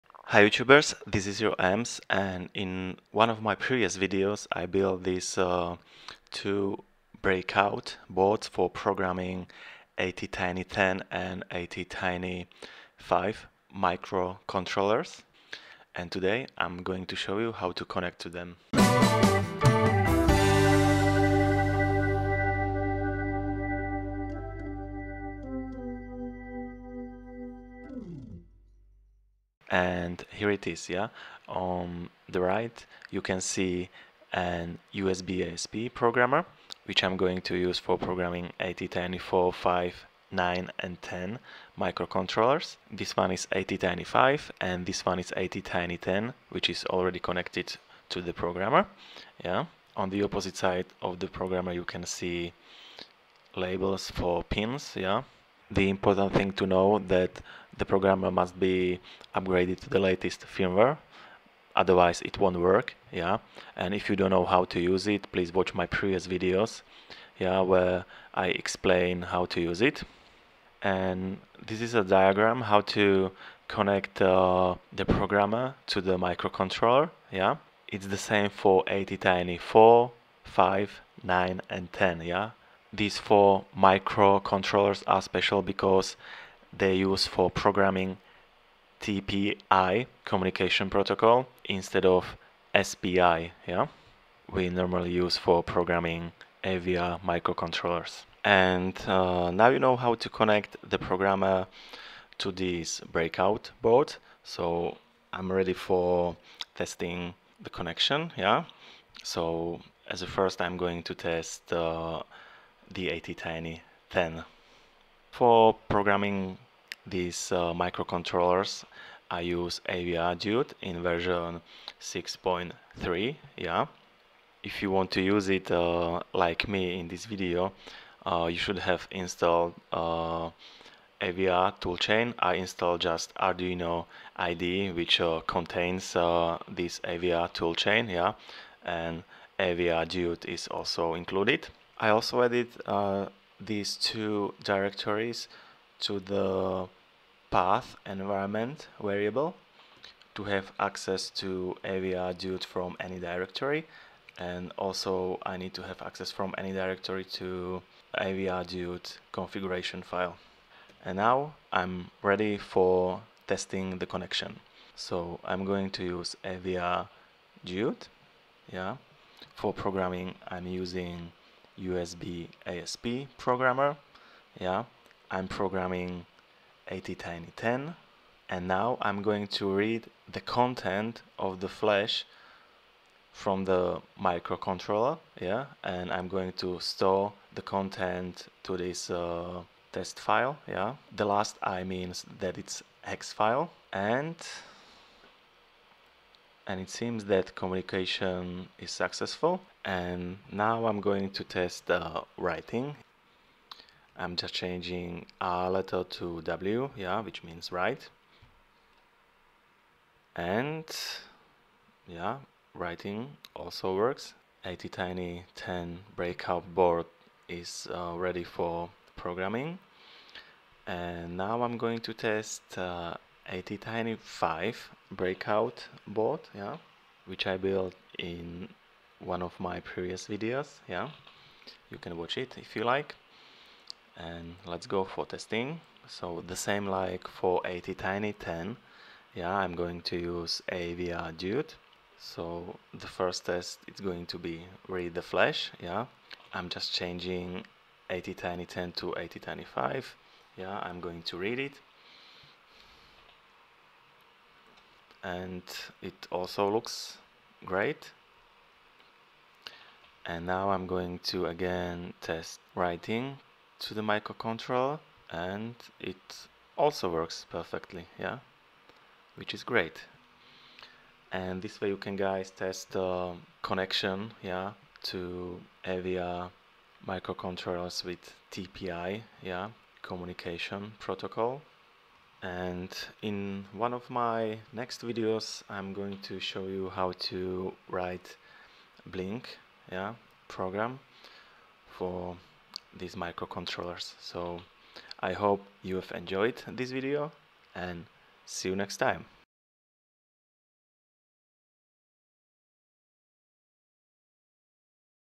Programming ATtiny10 with USBasp Programmer
This is a video about programming ATtiny10 AVR 8-bit microcontroller using an USBasp programmer ordered on AliExpress. The same way you can program ATtiny9, ATtiny5 and ATtiny4 which belong to the same family. In the video I show you how to connect the USBasp programmer to an ATtiny10 microcontroller and how to check the connection using avrdude utility. If you also want to use USBasp programmers for programming ATtiny10 you should upgrade them to the latest firmware which supports TPI programming interface necessary for programming these microcontrollers,

🛠 Tools which I use in the video 🛠
➟ ATtiny10
➟ SOT23/SOP10 to DIP10 Adapter
➟ USBasp Programmer
➟ 10-Pin to 6-Pin Adapter
➟ 10-Pin Ribbon Cable
➟ Small Breadboards
➟ Dupont Wires

Stay creative and keep on tinkering 😊